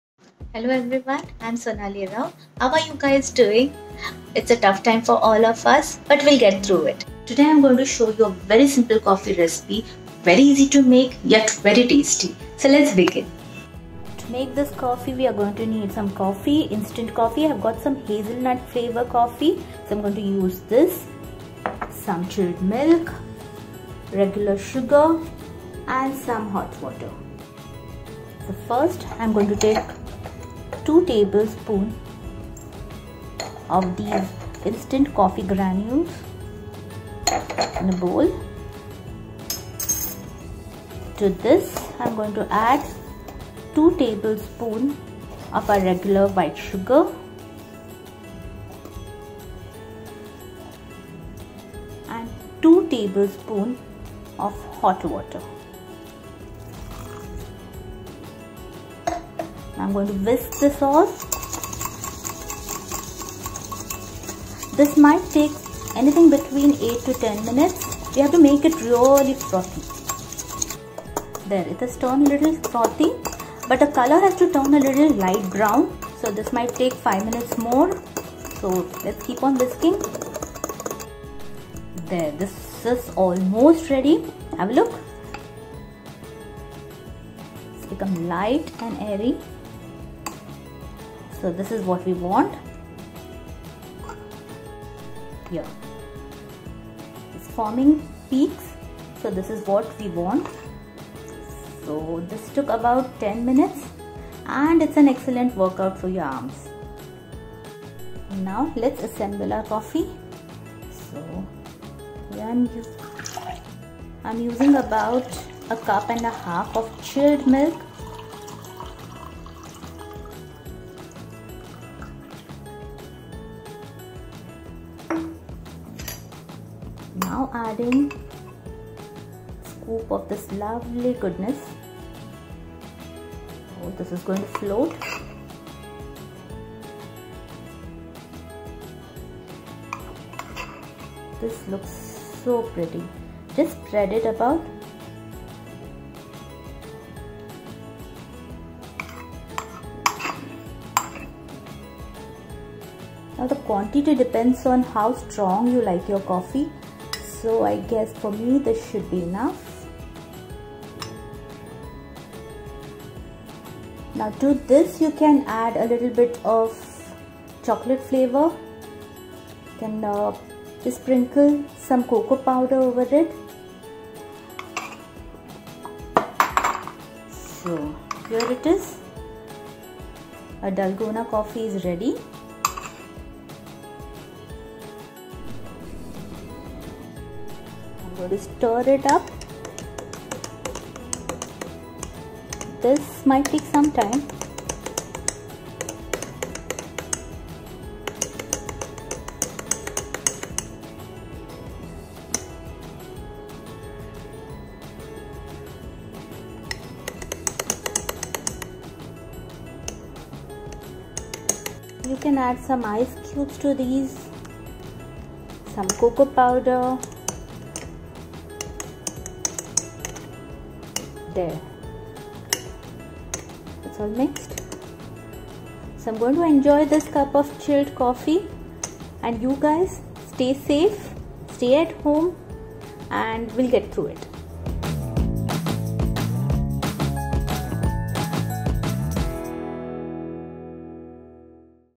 Quarantine Special Dalgona Coffee by Sonali Raut | Trending Coffee On Social Media | Lockdown
Now that we all have to stay home for quite some time, here's a simple coffee recipe by Sonali Raut! 'Dalgona coffee' is trending on social media. It's very easy but the perfect coffee to try out during isolation times. Stay home, stay safe guys!